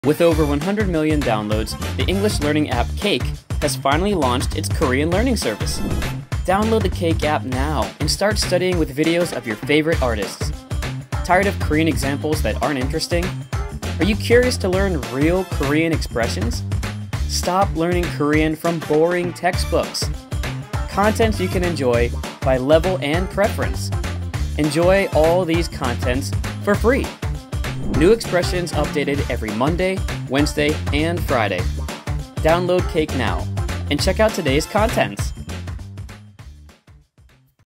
🚨드디어 방탄소년단의 영상으로 한국어 공부를 할 수 있는 앱 출시🚨
“I hope they would release an education app. I would really love to learn Korean.”
“Are they going to release a new app? Then that would be cool.”

드디어 방탄소년단의 영상으로 한국어 공부를 할 수 있는 앱이 출시되었어요!
이제 365일 케이크에서 재밌게 한국어 공부를 해볼까요?

📍Today
매일매일 업데이트되는 표현 영상! 핵심 표현(Key Expression)은 반복 듣기를 하면서 직접 따라 해보세요.
✔ 방탄소년단 콘텐츠는 매주 월/수/금 업데이트 됩니다.

📍Class
보다 체계적으로 한국어 학습을 하고 싶다면?
나에게 맞는 레벨을 선택하고, 한국어를 차근차근 배울 수 있어요.
✔ 앱에서 한글을 직접 써보고, 게임처럼 퀴즈도 풀다 보면 한국어 실력이 쑥쑥!

📍Speak
한국어 문장을 보고, 듣고, 원어민처럼 말할 수 있도록 Cake의 AI가 어색한 발음을 짚어 줄 거예요.
HYBE EDU의 BTS Learn! KOREAN 시리즈 및 BLACKPINK IN YOUR KOREAN의 내용도 앱에서 만나볼 수 있어요.

📍Snack
매일 가볍게 재밌는 콘텐츠를 보고, 한국어 공부도 해요!
✔ Today와 Snack에서 중요한 문장은 저장 버튼 누르고, Review 탭에서 다시 확인!

지금 바로 Cake 앱을 다운받으시고, 오늘의 콘텐츠를 확인해보세요!